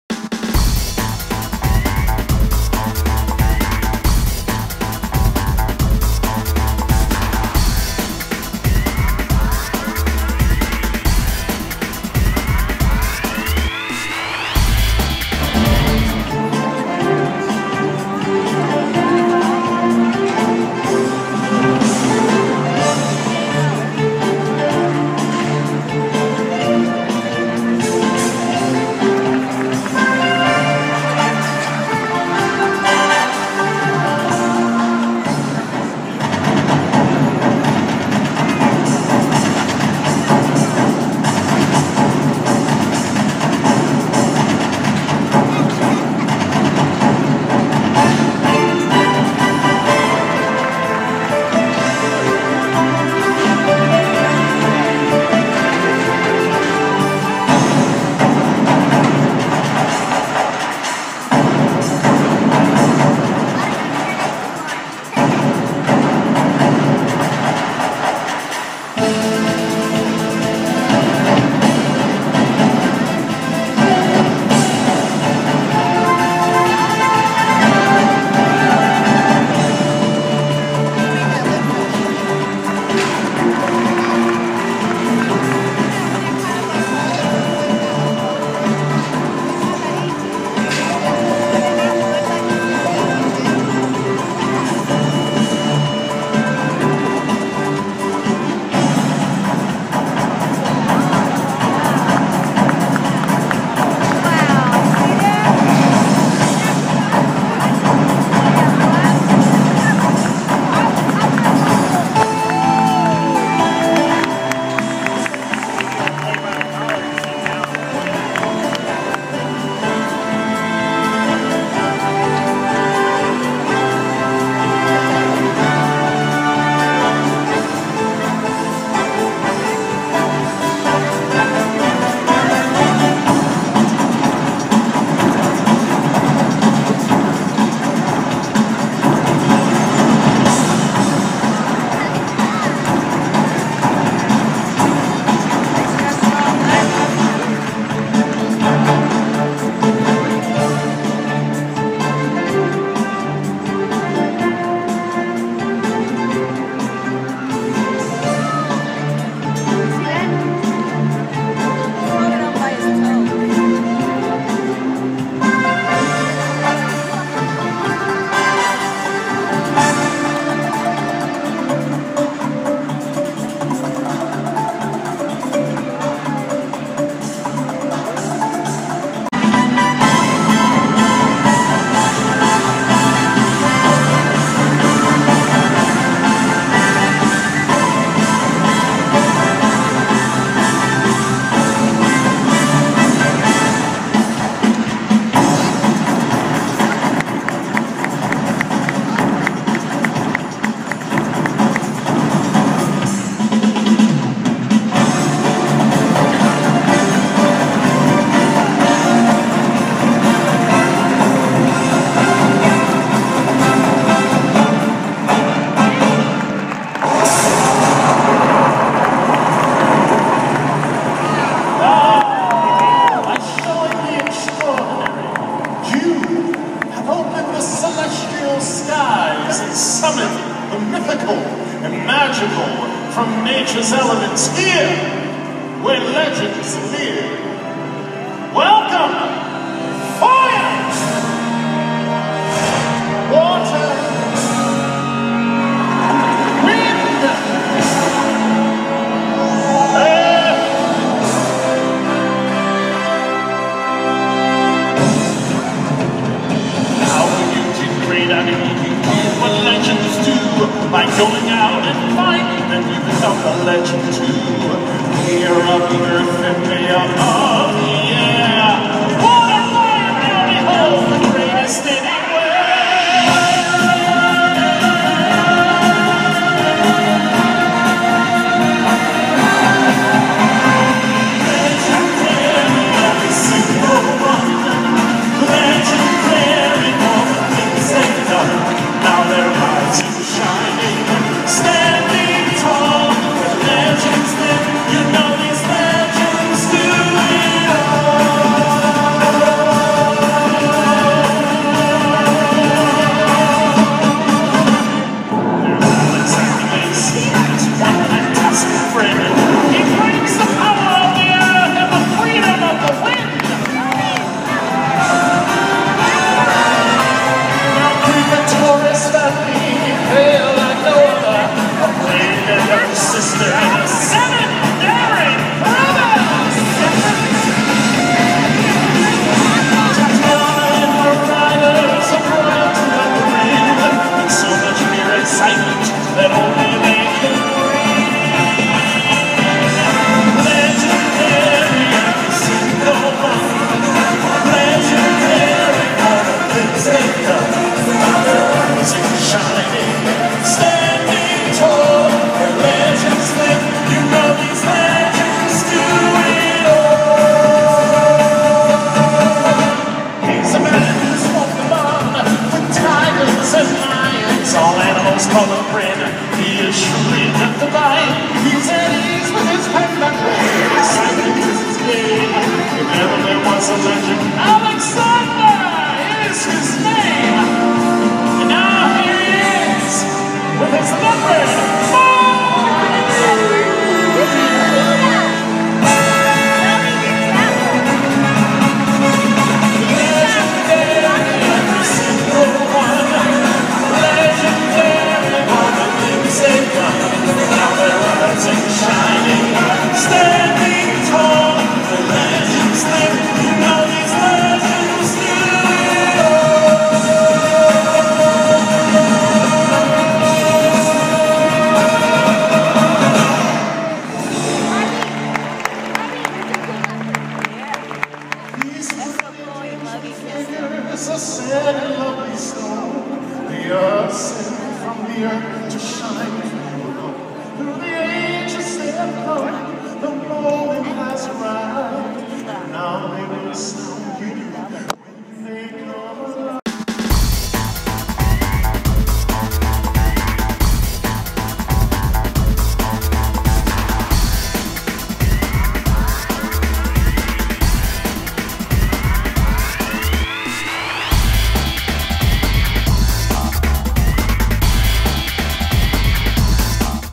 RINGLING BROS. AND BARNUM & BAILEY CIRCUS:Legends Pole Climbing
RINGLINGBROS. AND BARNUM & BAILEY CIRCUS  Pole Climbing Dance